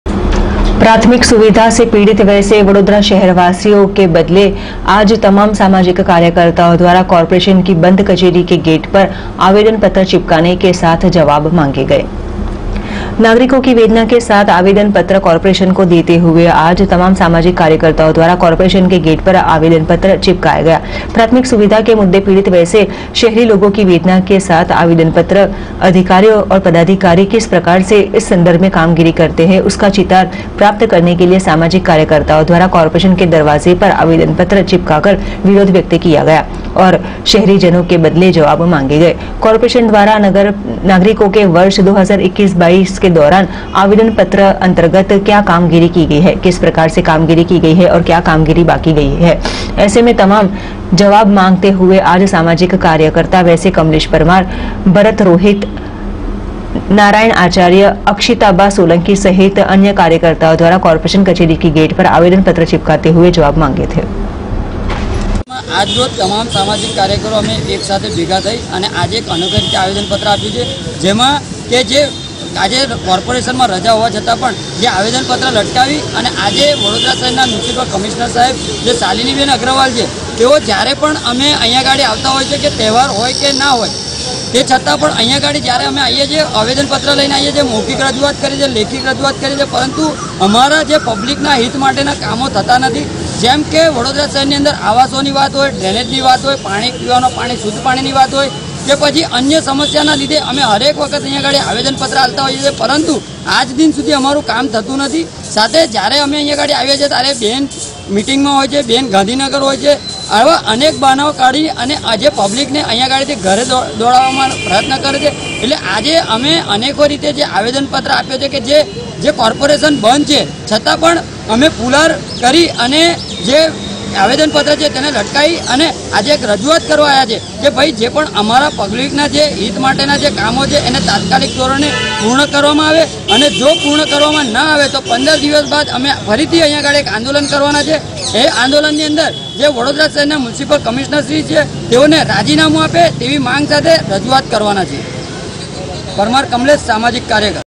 ibn news vadodara
शहेरीजनों के वेदना को व्यक्त करते हुए तमाम सामाजिक कार्यकरो द्वारा कॉरपोरेशन कचहरी के गेट पर लगाया गया आवेदन पत्र